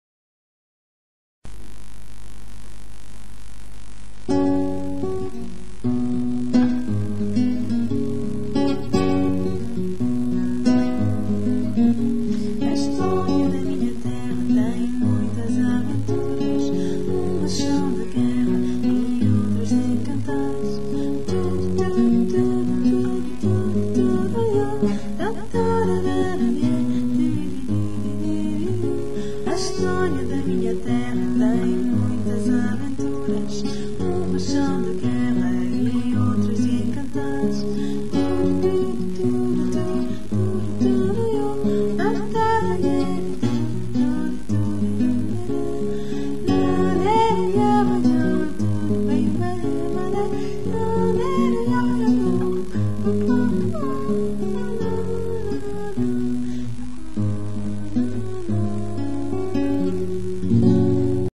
My music A5
this music i wrote when i was a little girl. Its in portuguese and it means: " the story of my land has many adventures, some of war and others of peace"